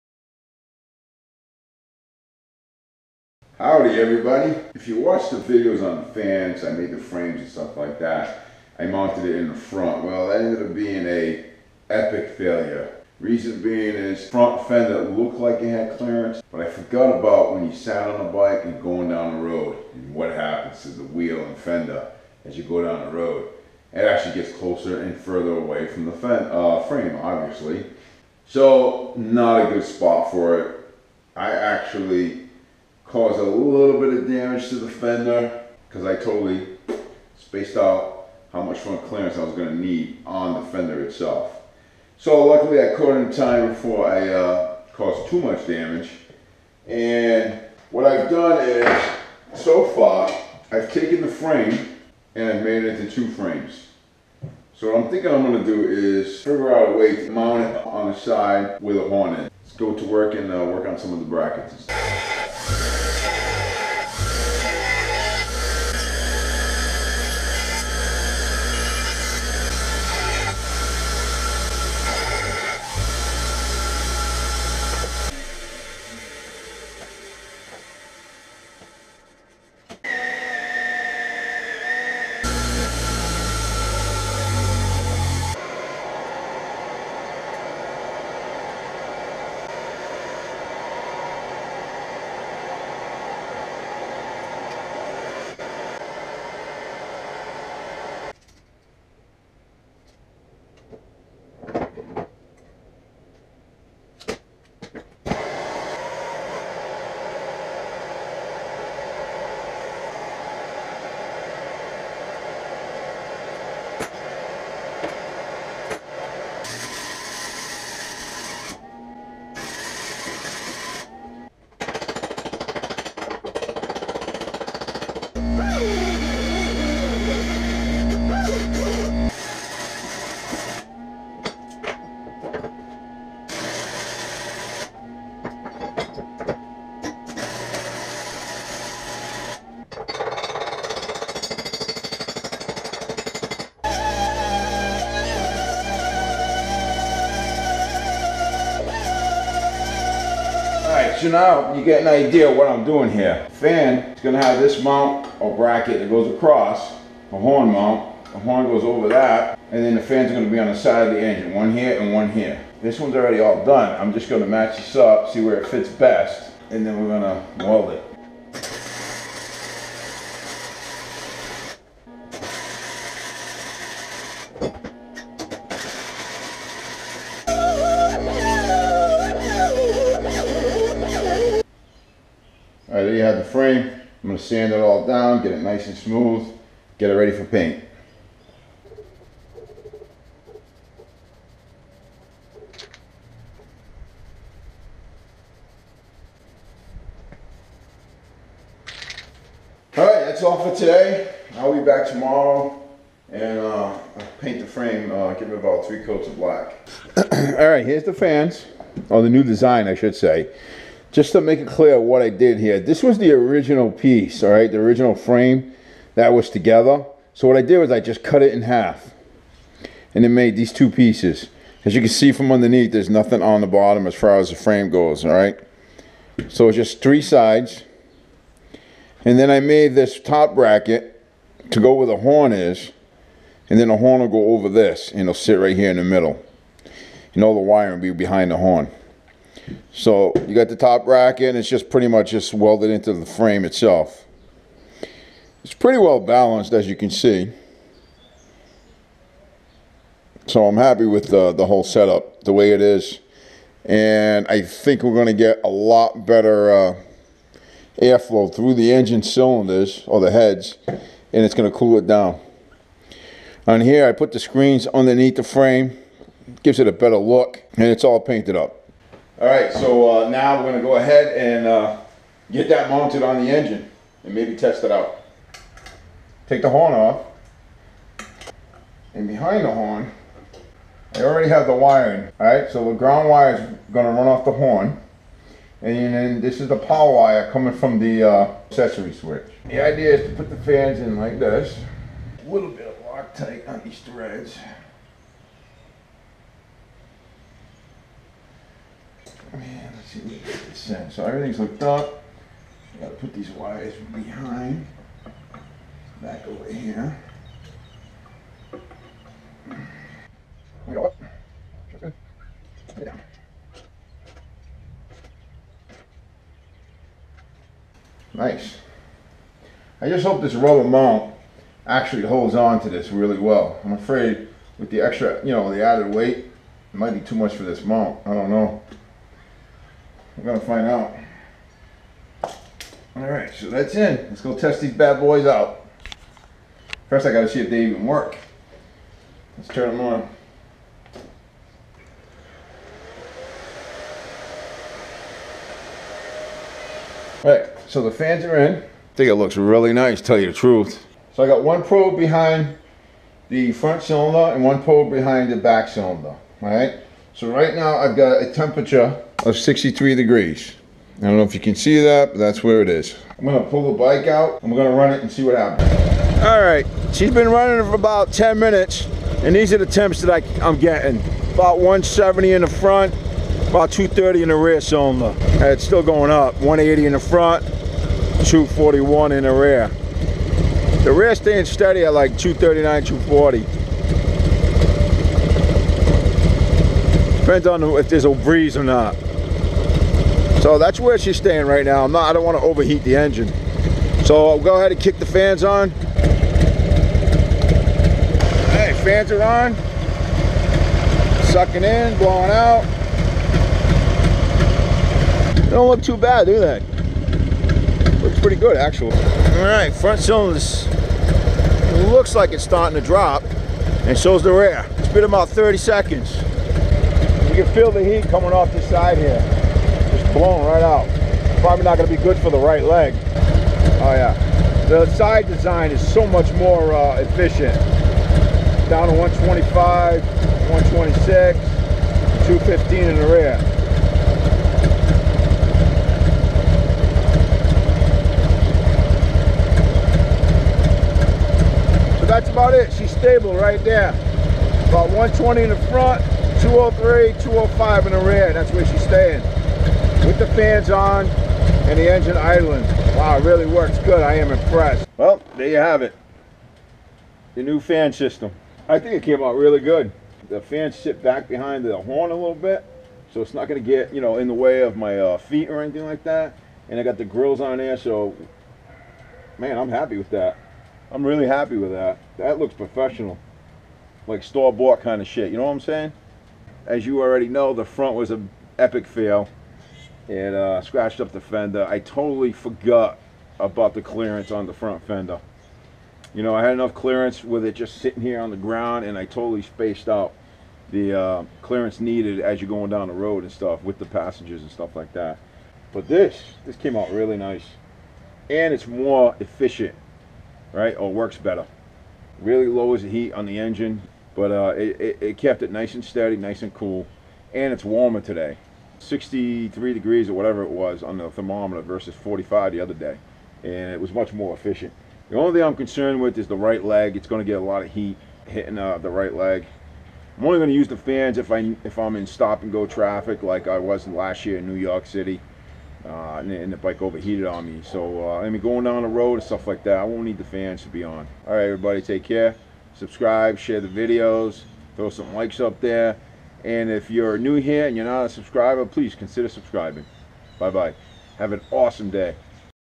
Harley Davidson Engine Cooling Fans- DIY Part 3 | Techn' Moto
Harley Davidson Engine Cooling Fans- DIY Part 3

Finally I found a fan configuration that works and works well. After the first design became a complete failure because of the clearance issues I decided to attempt a side engine mount design and came up with this contraption. Not only does it perform well but also looks good too.